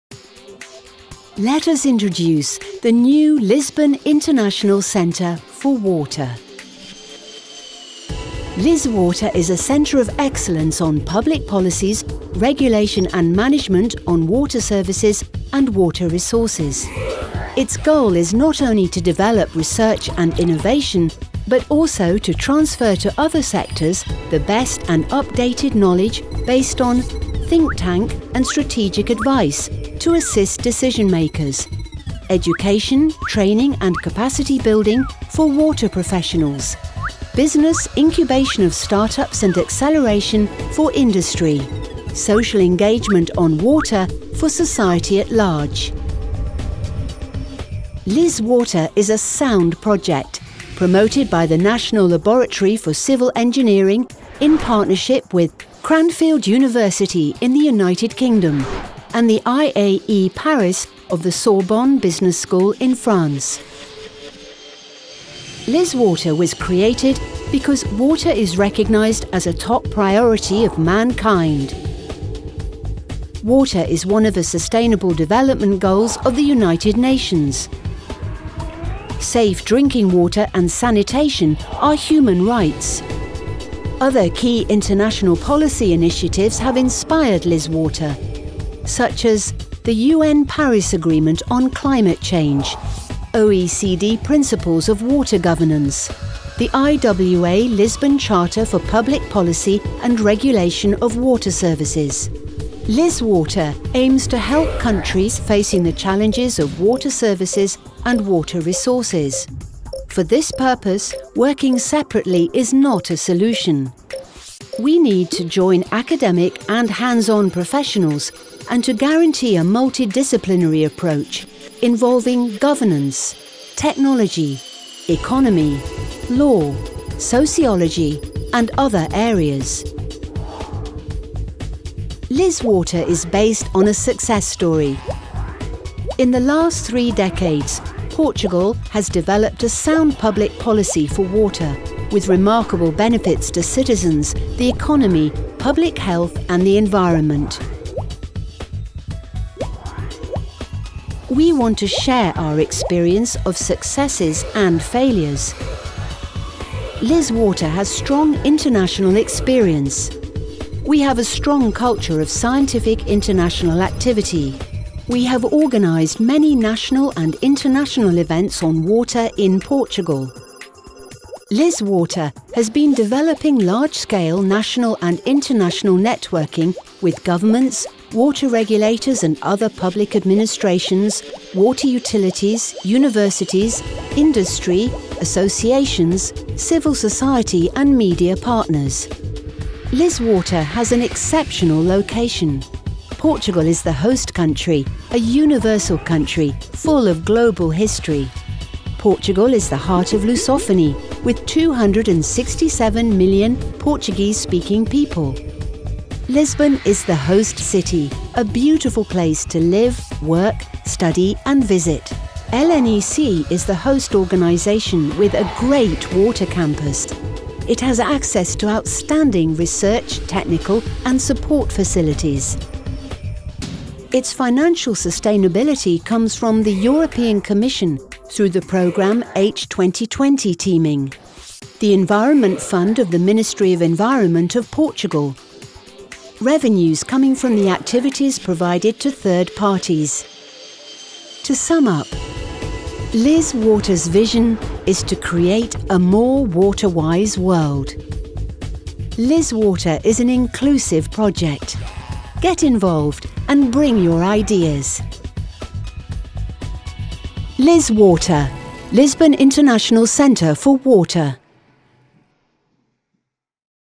LIS-Water - Lisbon International Centre for Water
Find out more about the LIS-Water Centre in this 4 minute video!

LIS-Water will be the first international centre for water in Portugal, dedicated to public policies, regulation and management related to water resources.